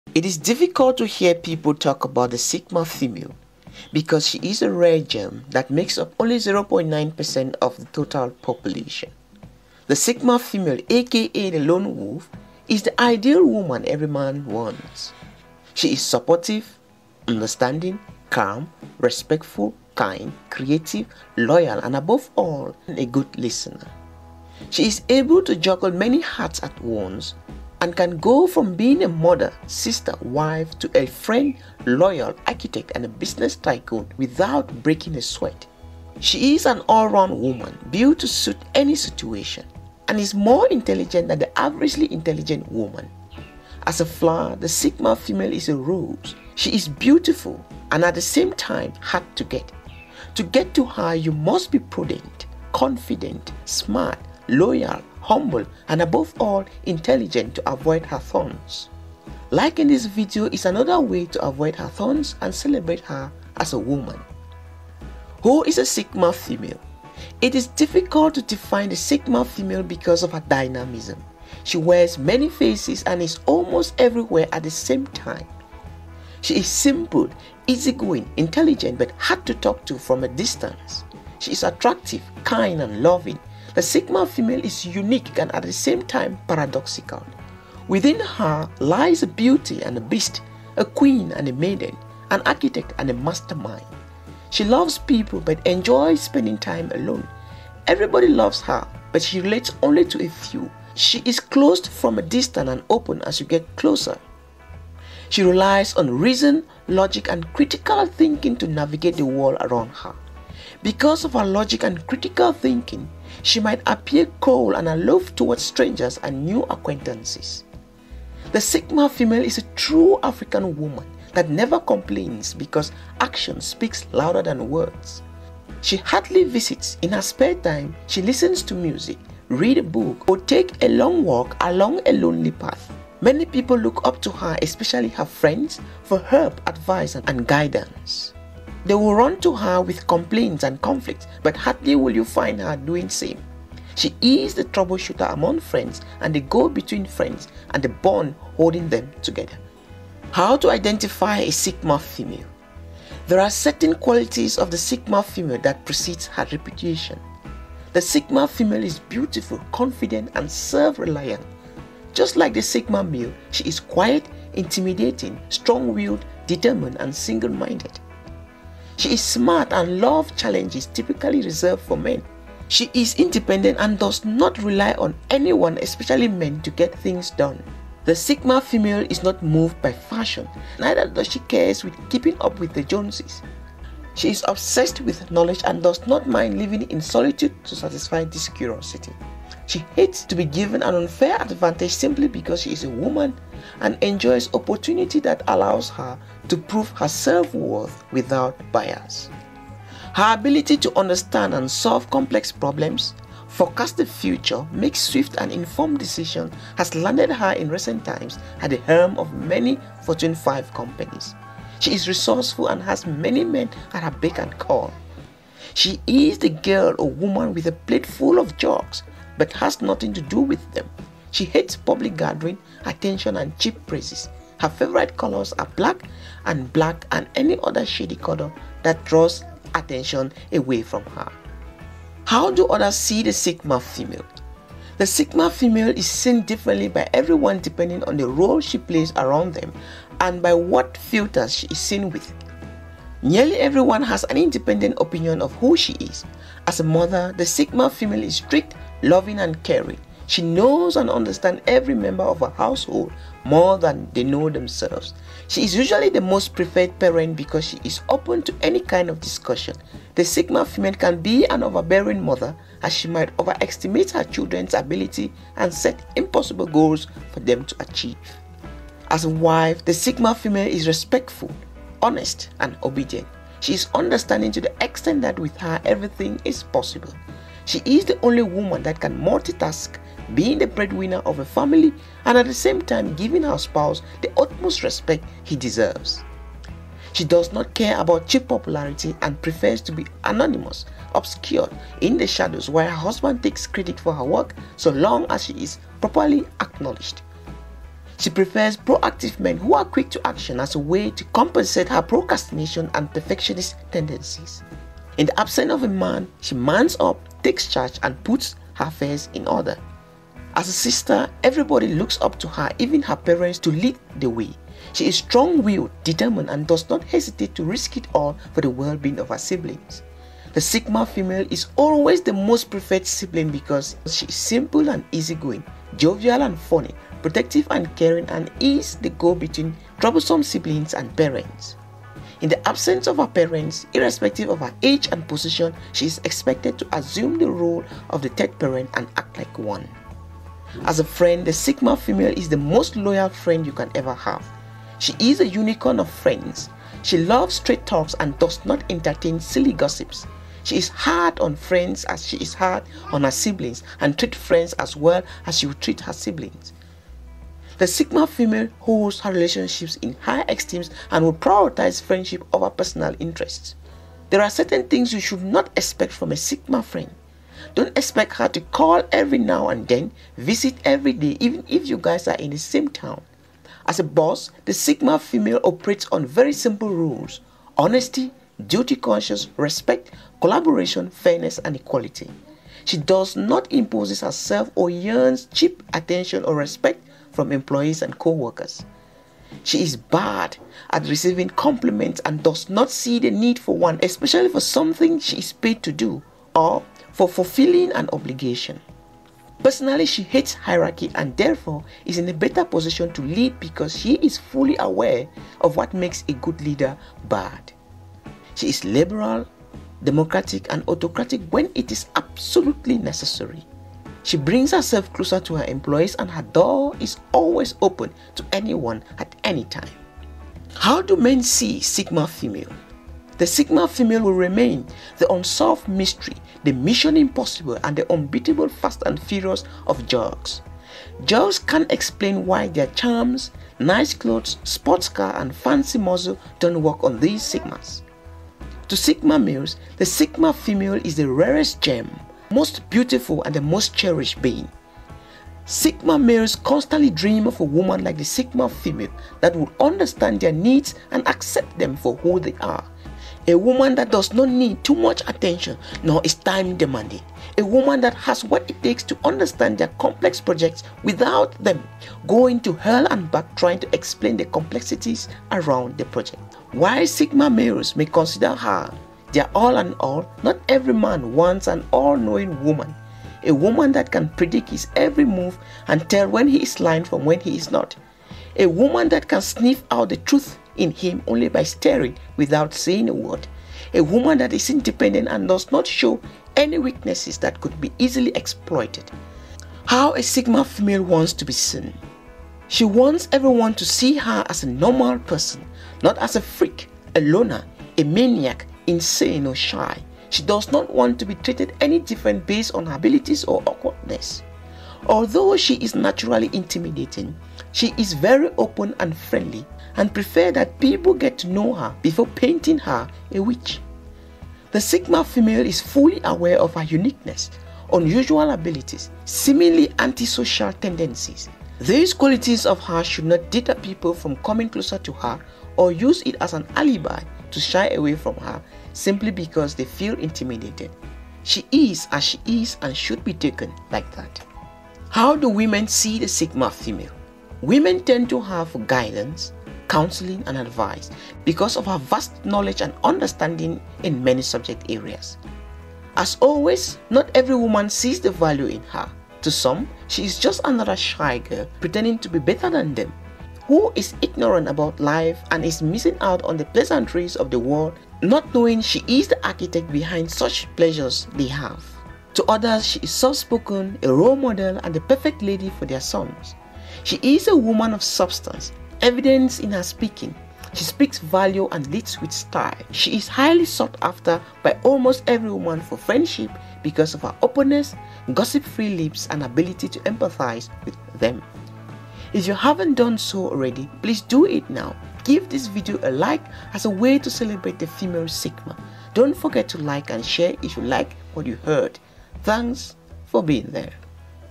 Sigma Female--The Lone Wolf
It is difficult to hear people talk about the sigma female because she is a rare gem that makes up only 0.9% of the population. The sigma female AKA the lone she-wolf is the ideal woman everyman wants. She is supportive, understanding—calm, respectful—kind, creativity—loyal, and a good listener.